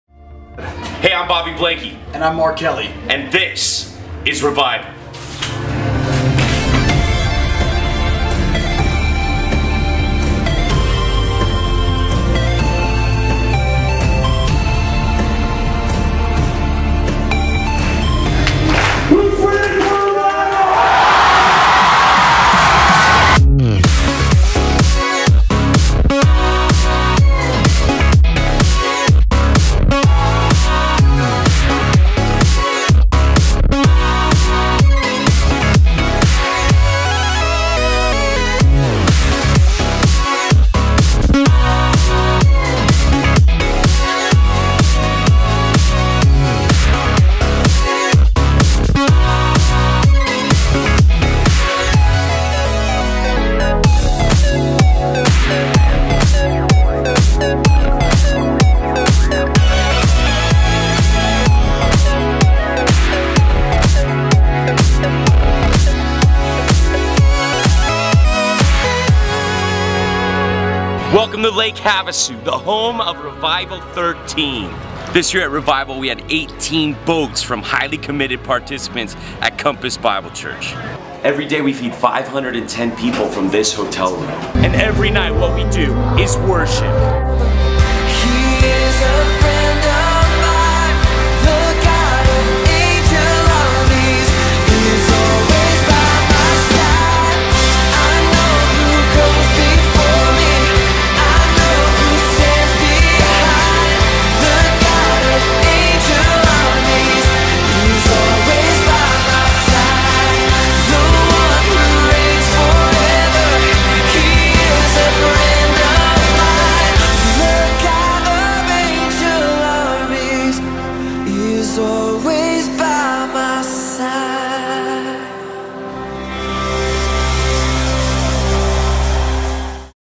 Revival: The Invisible War (2013)
Recap for Revival 13. Duck Dynasty, Inflatable Ducks, and Mythical Creatures. (July 29, 2013). ‎